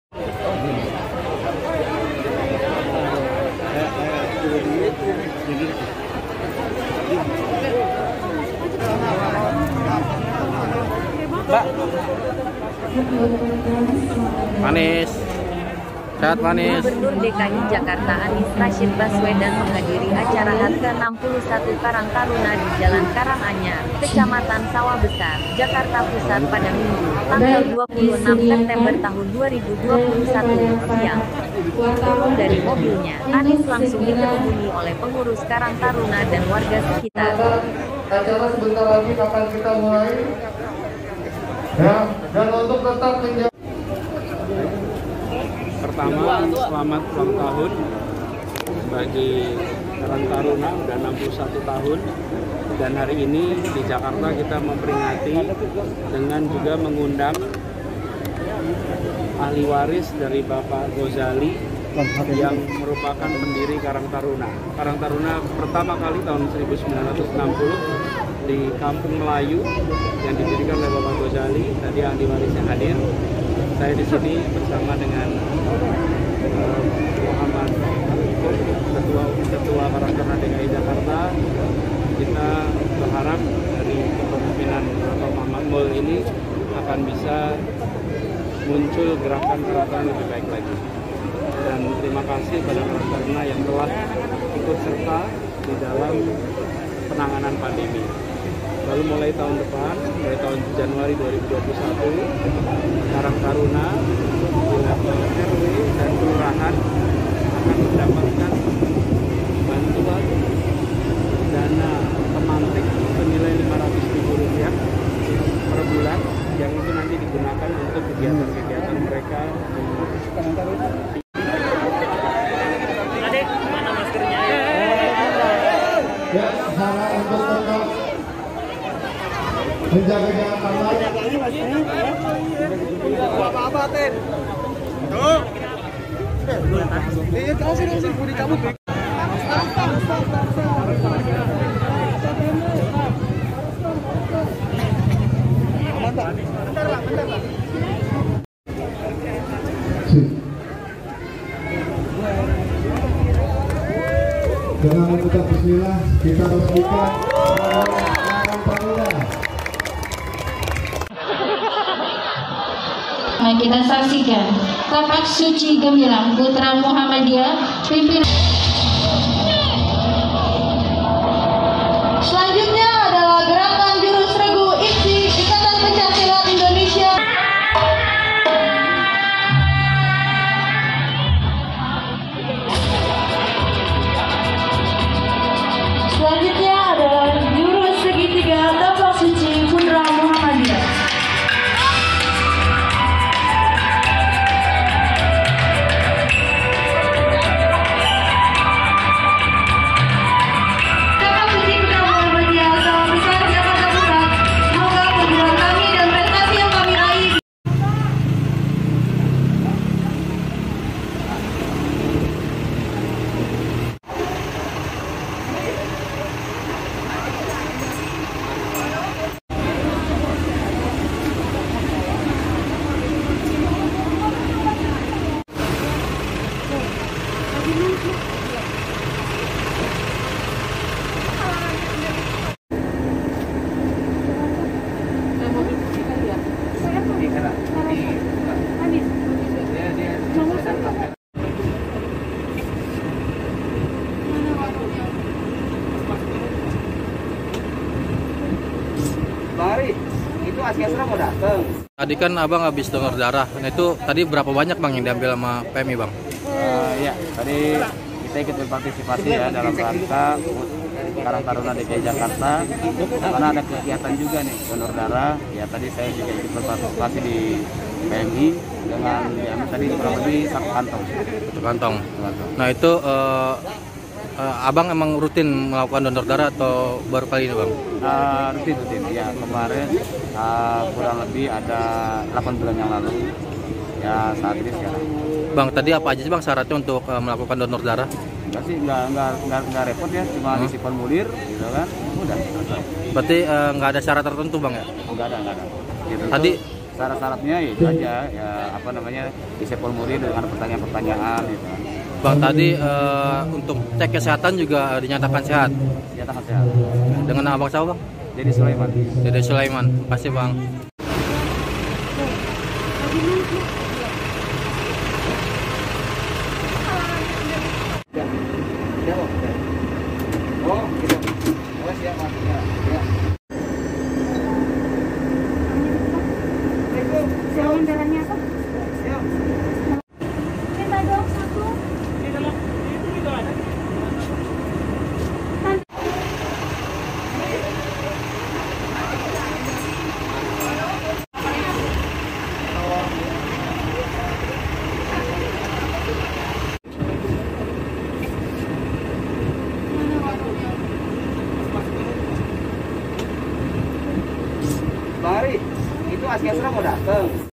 Anies Minta Karang Taruna DKI Jakarta Tetap Berikan Manfaat untuk Masyarakat
anies Baswedan#HUT Ke-61 Karang Taruna# donor darah

Gubernur DKI Jakarta Anies Rasyid Baswedan menghadiri acara HUT ke-61 Karang Taruna di Jalan Karang Anyar, Kecamatan Sawah Besar, Jakarta Pusat pada Minggu (26/9/2021) siang.